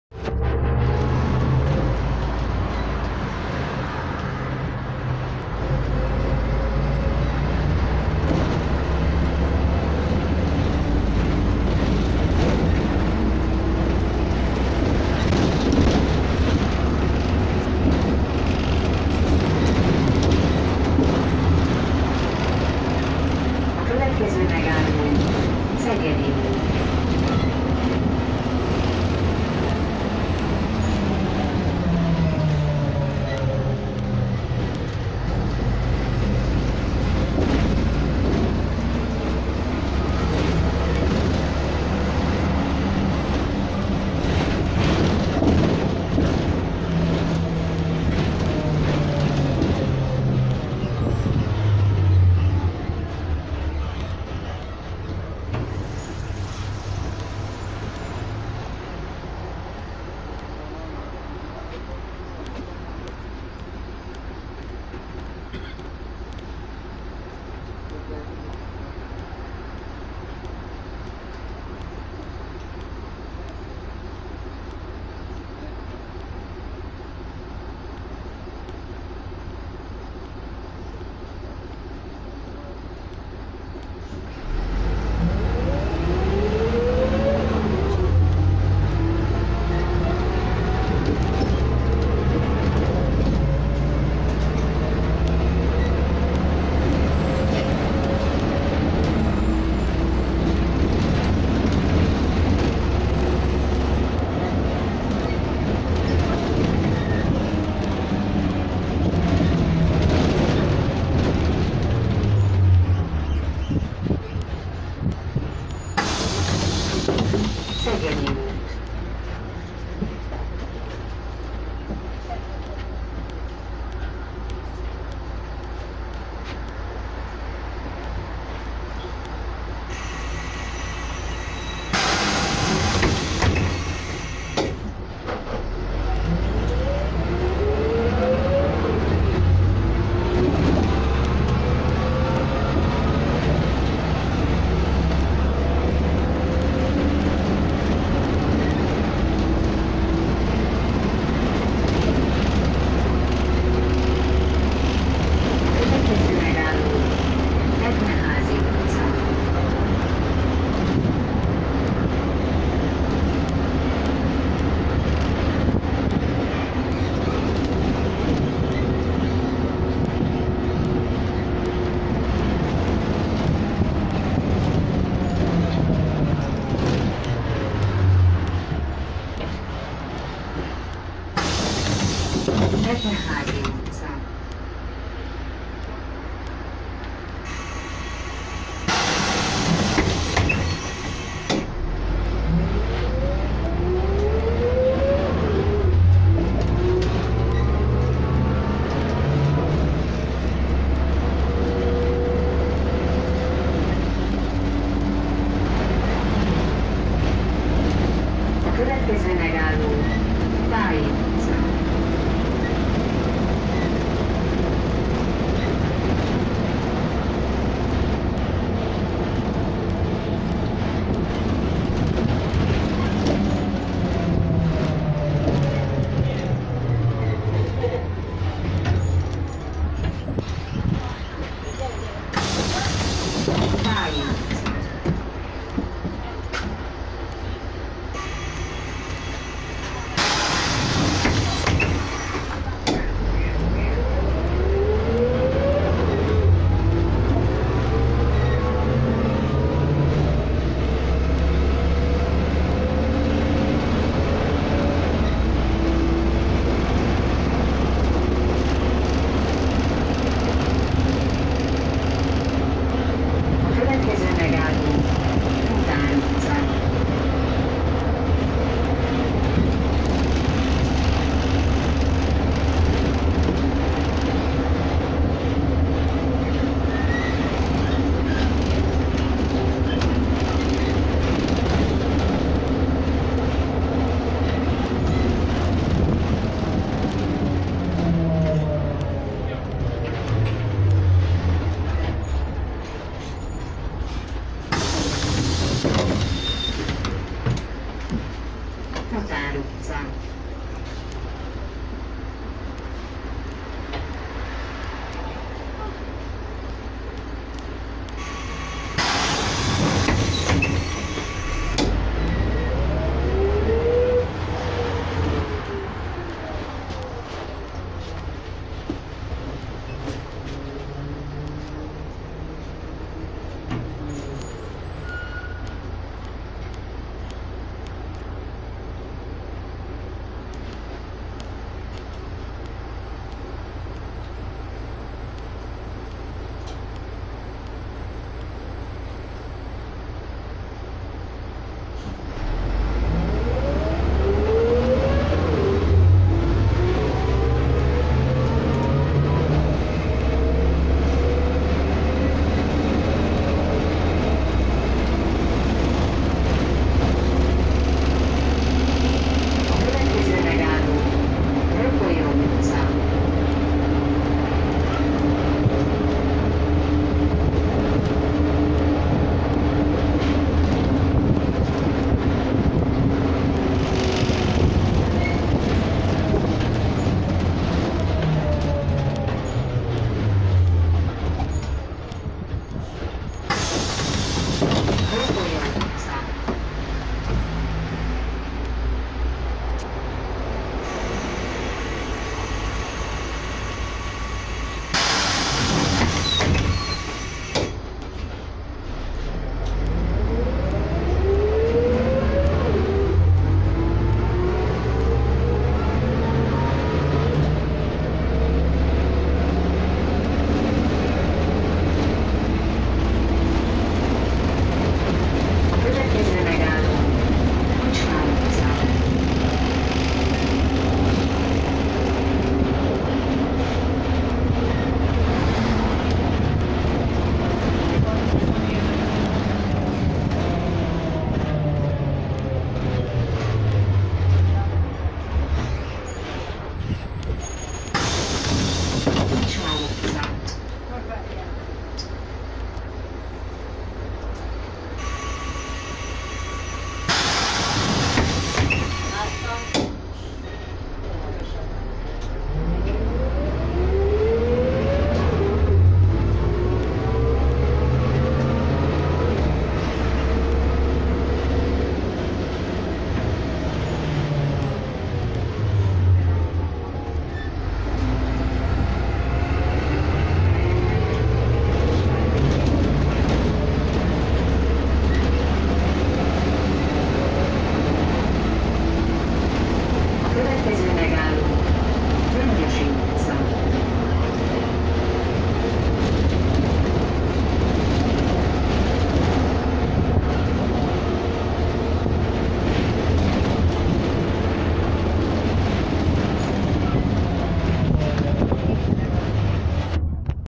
IKARUS 280 BPI-973 BKV BKK UTAZÁSA 30A VONALON DE AZÓTA SELEJTEZVE LETT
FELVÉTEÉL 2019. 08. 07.
VISZONYLAT 30A BUSZ
TELEPHELY CINKOTAI AUTÓBUSZGARÁZS
KNOR UTAS FELMÉRÉS